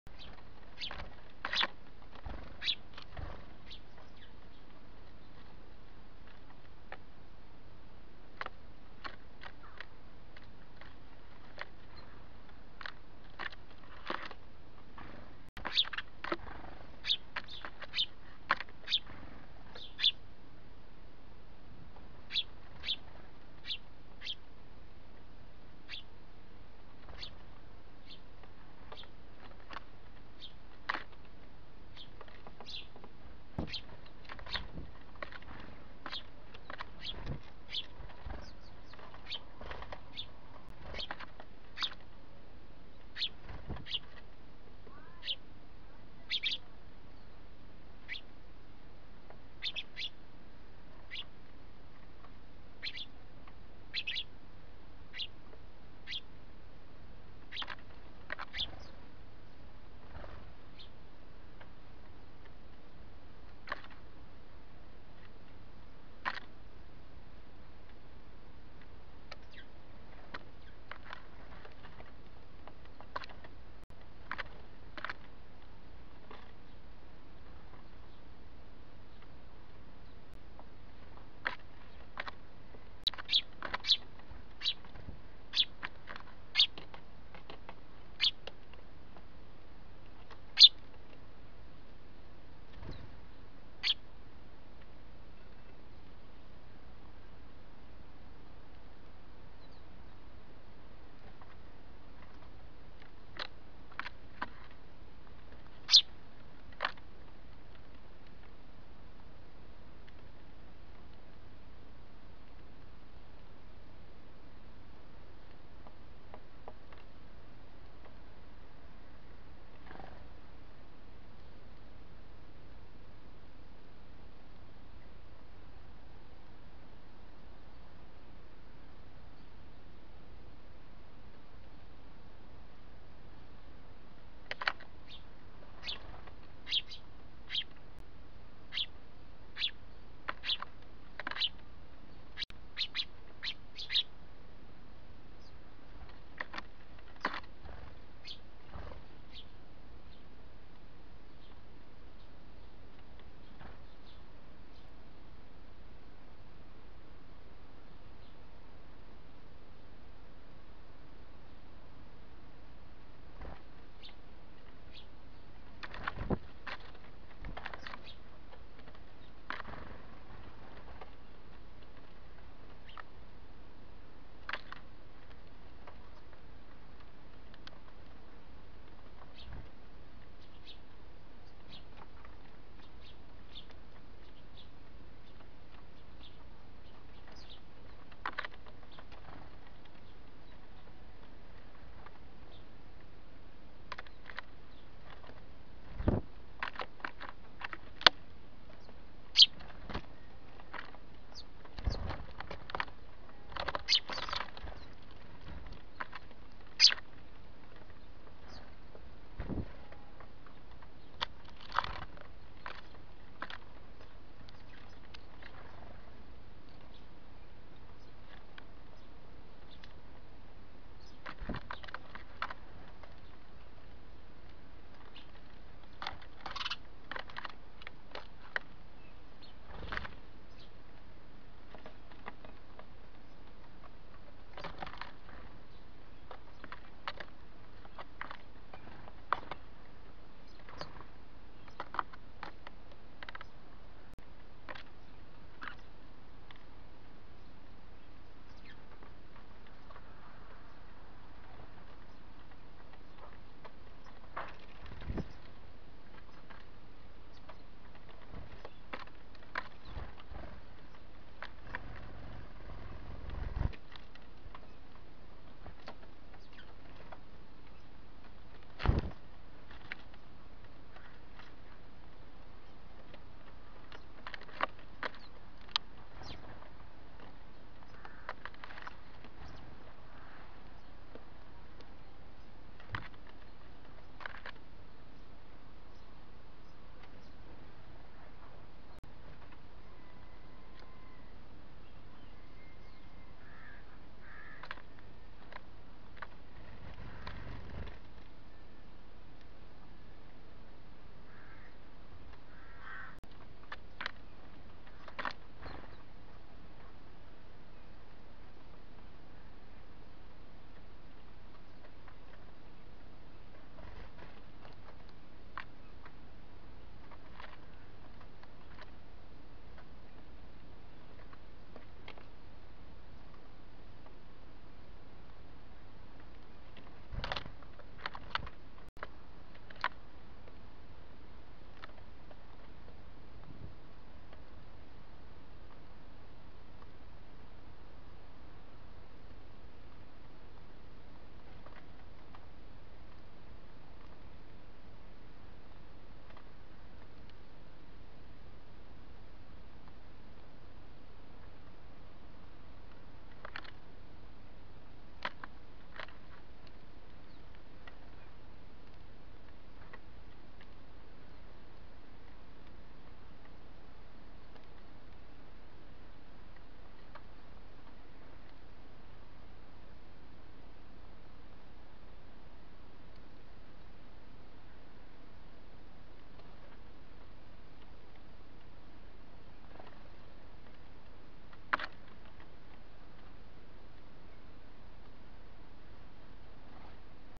Boids
Sparrows at the feeder in Portballintrae, 16th June 2024.